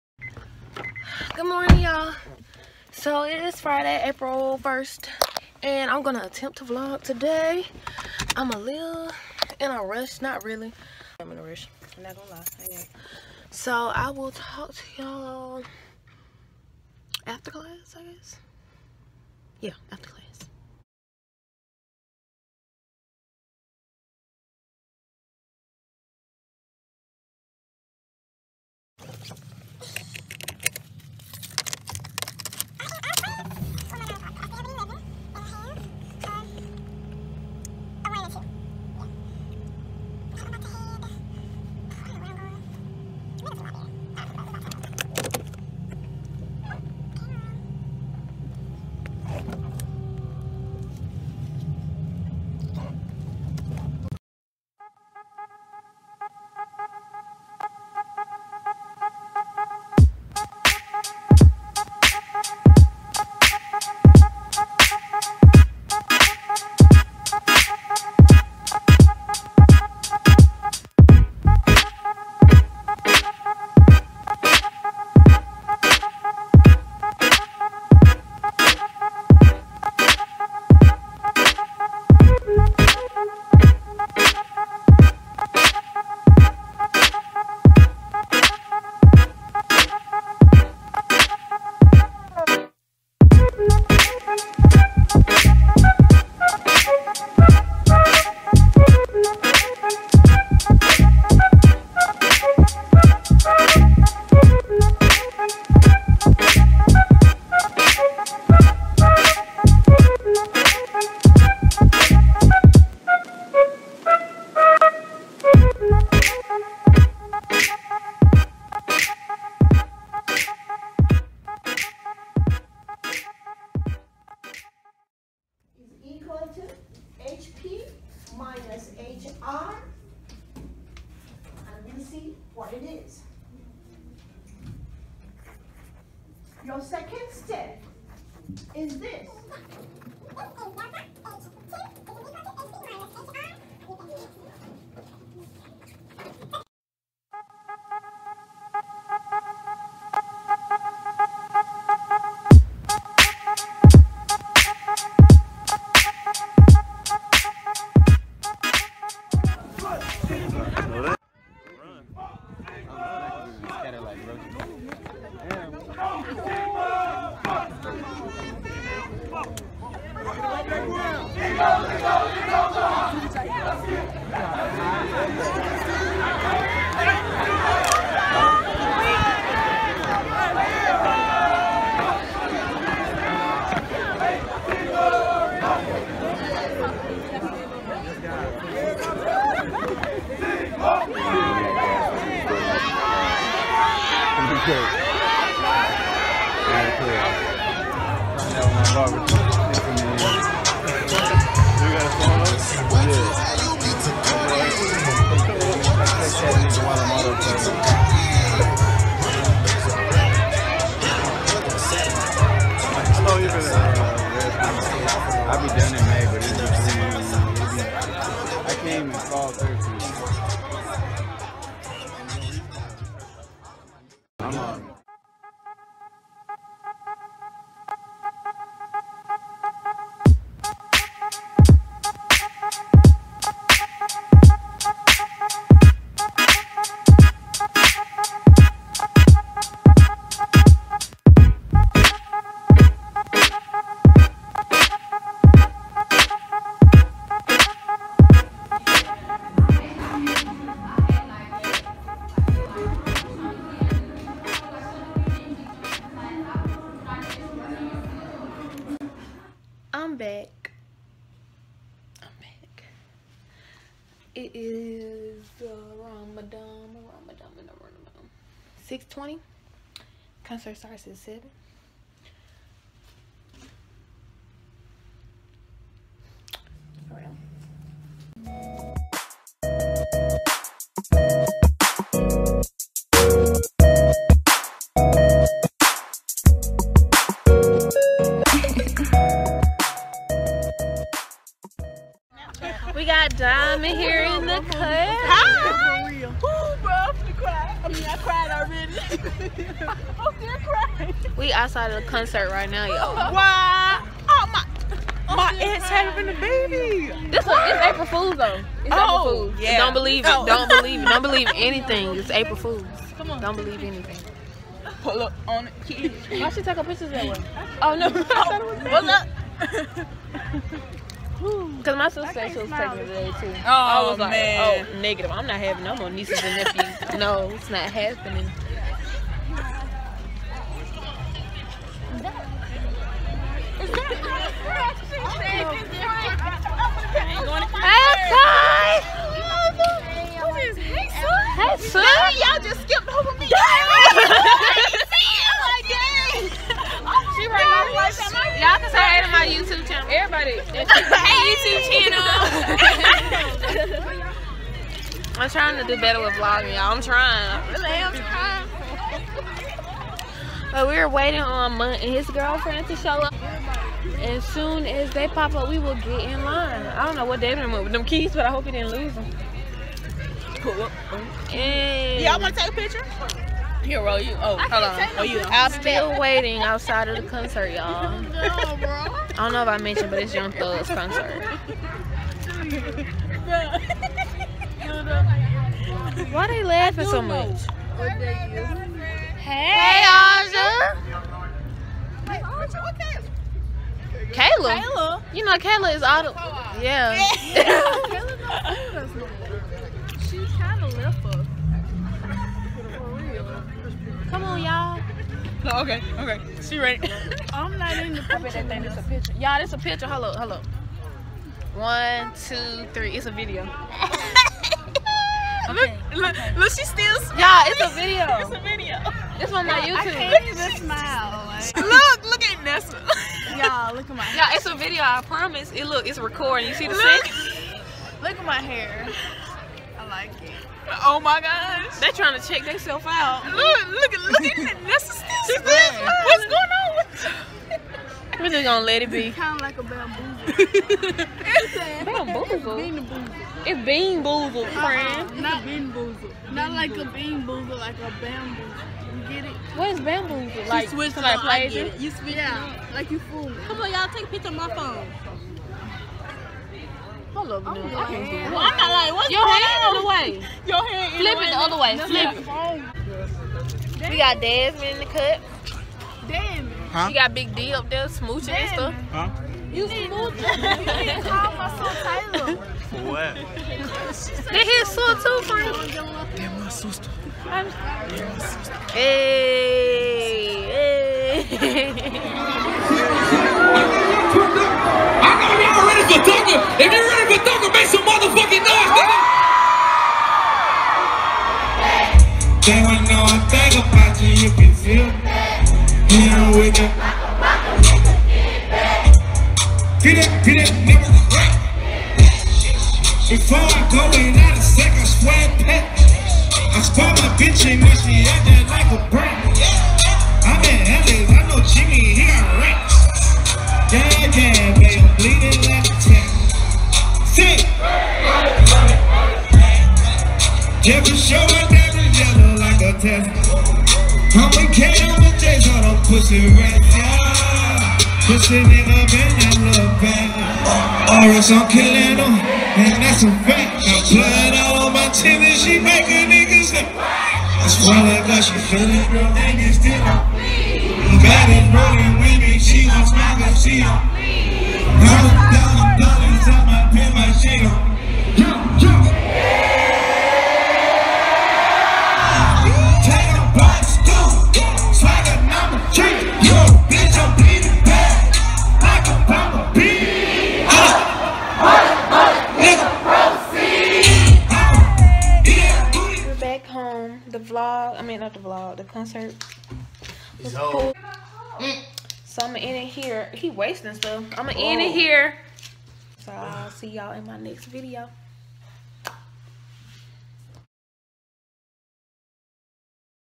Vlog #1 | Class, Probate, Young Thug Concert | College Edition
The Final Day of Spring Week On The Hill!